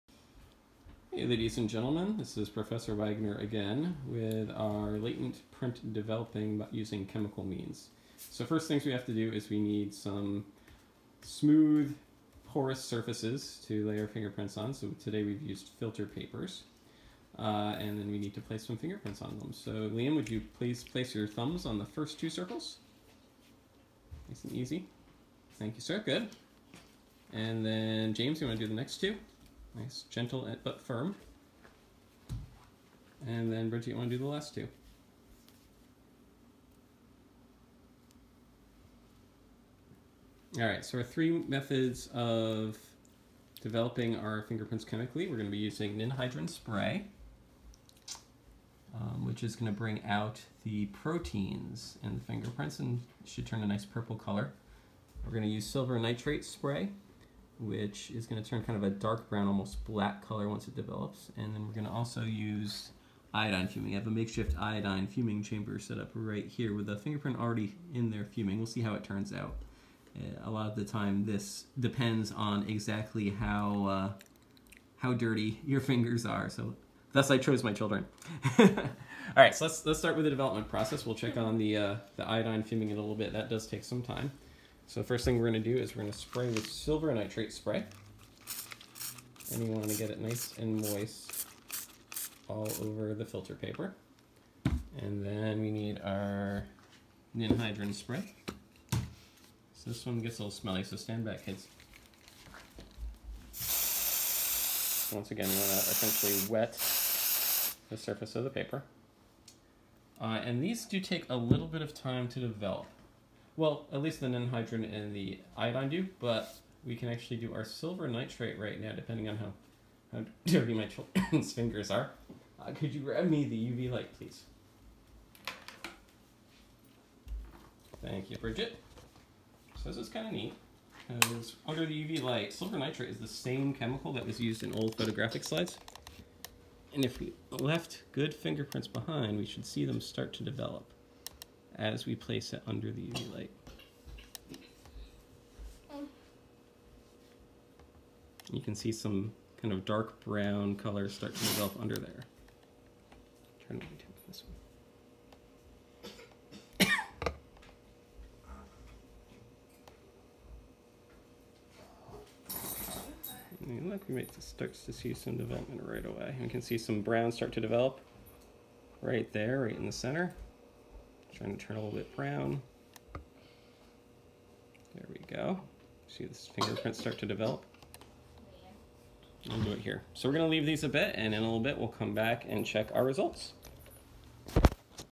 Chemical Development of Fingerprints Demonstration Video
This video demonstrates the chemical development of fingerprints using silver nitrate and ninhydrin spray.